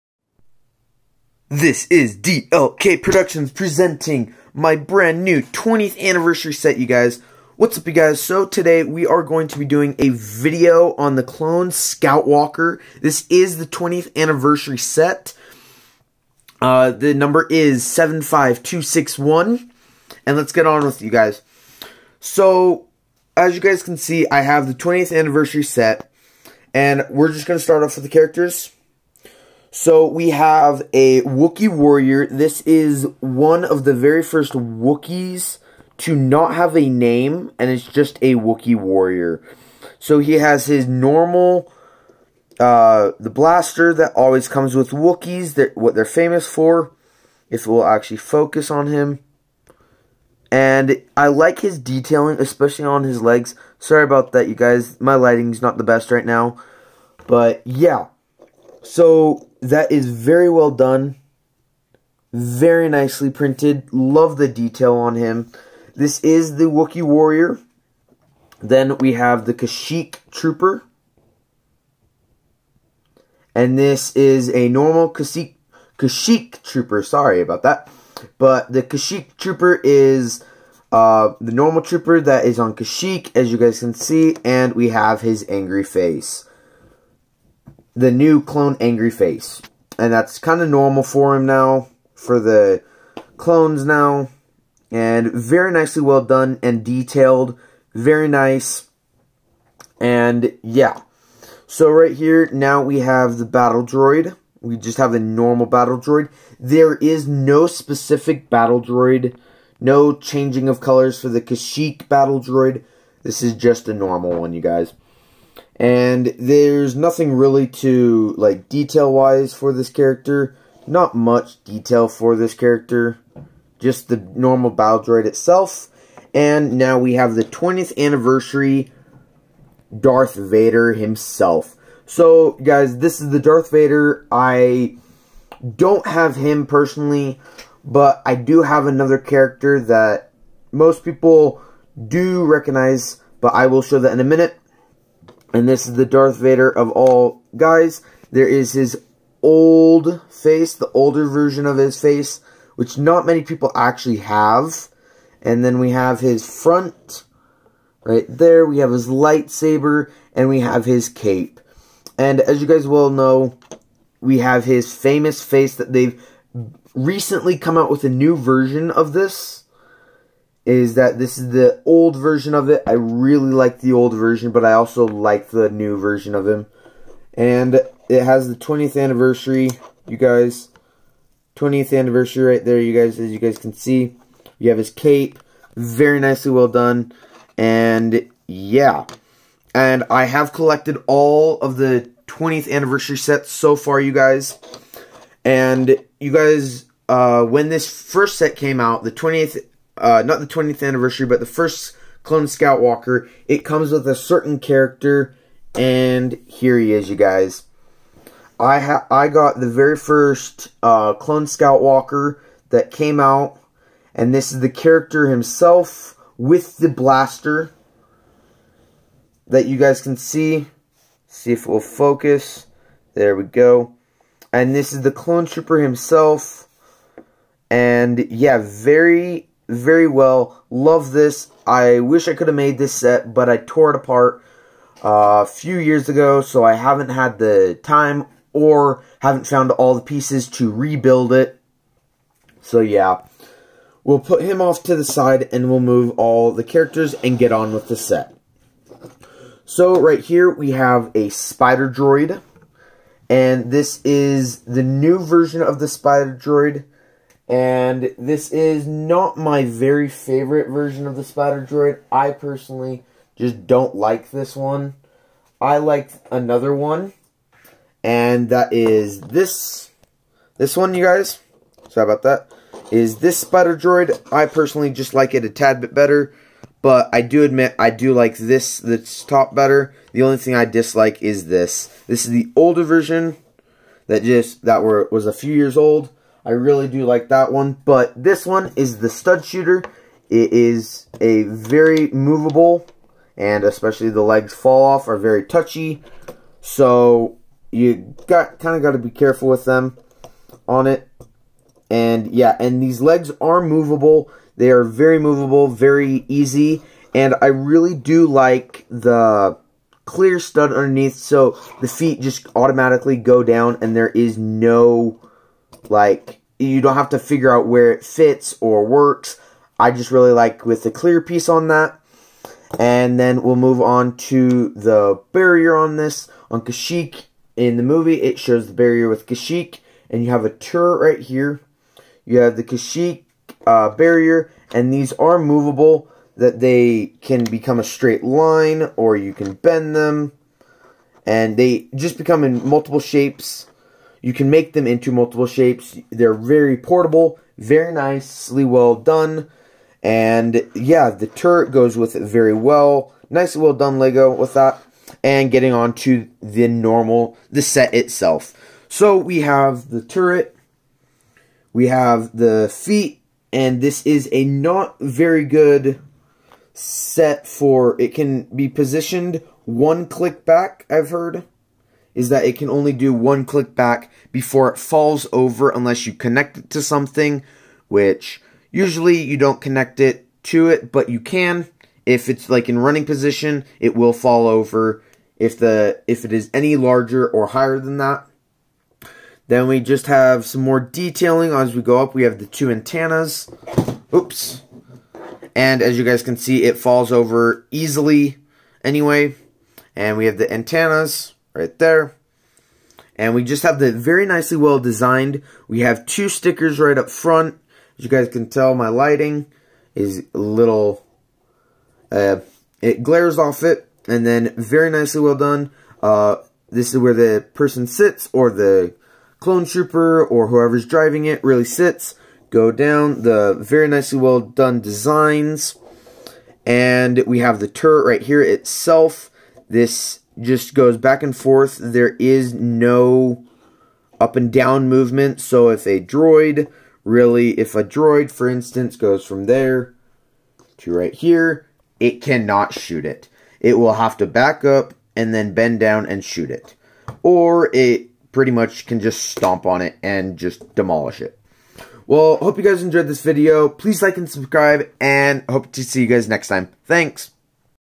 Lego 20th Anniversary Scout Walker Review!
Also i will be posting the 50 sub special video very soon ! I collected the whole series of the set as of so far. There is a little surprise in the video.